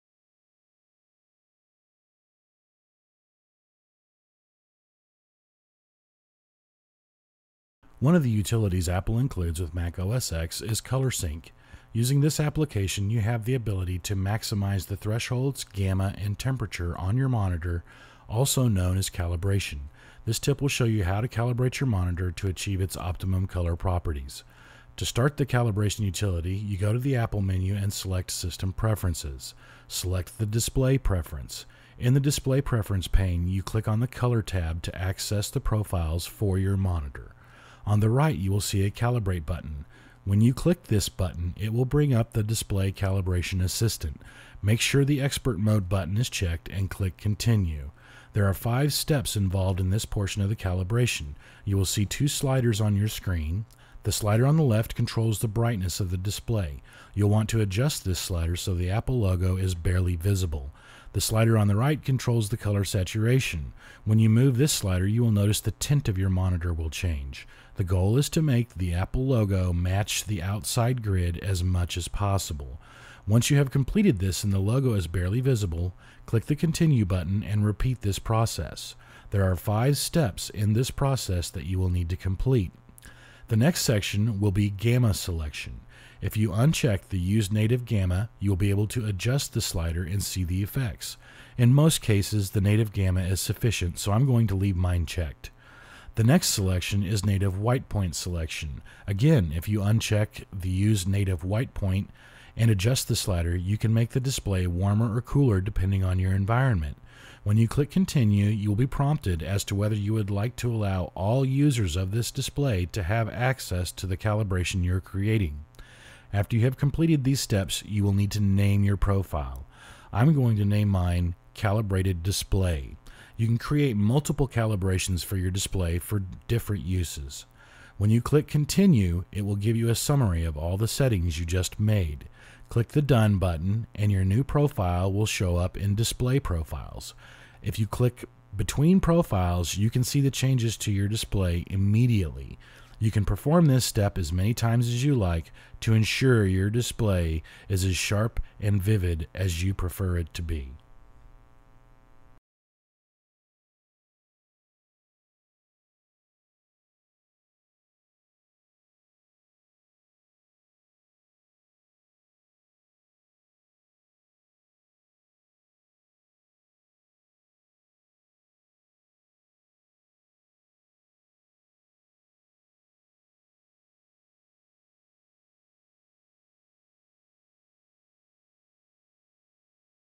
How To Calibrate a Mac Display
A video tech tip that shows you how to calibrate your Mac's display using Apple's Display Calibrator Assistant.